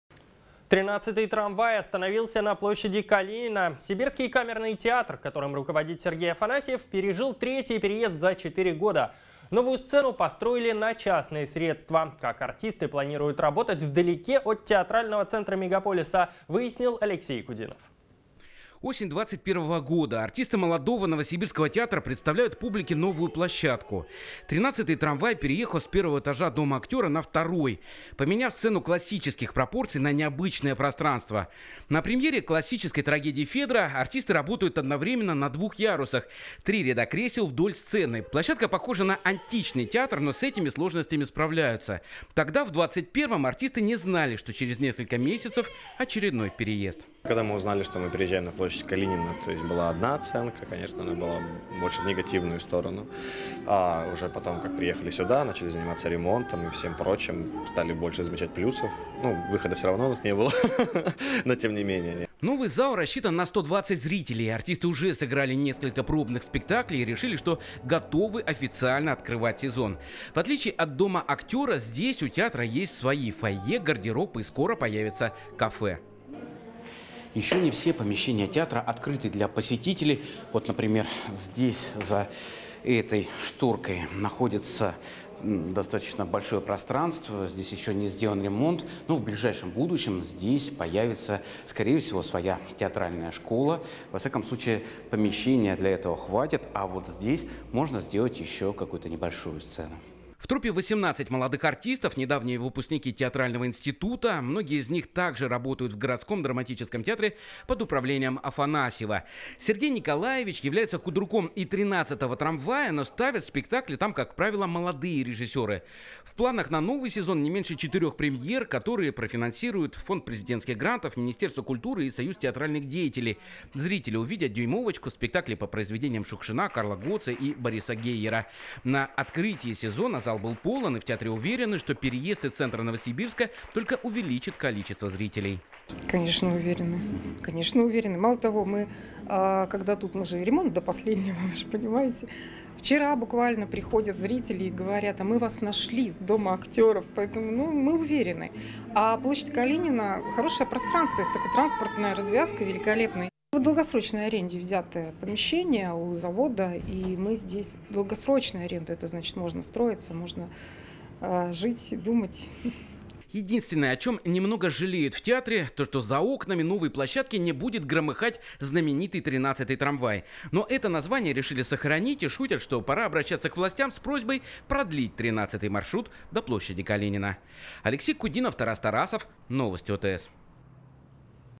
"13 трамвай" переехал из Дома актёра. Где теперь смотреть постановки?
"13 трамвай" остановился на площади Калинина. Сибирский камерный театр, которым руководит Сергей Афанасьев, пережил третий переезд за четыре года. Новую сцену построили на частные средства. Как артисты планируют работать вдалеке от театрального центра мегаполиса?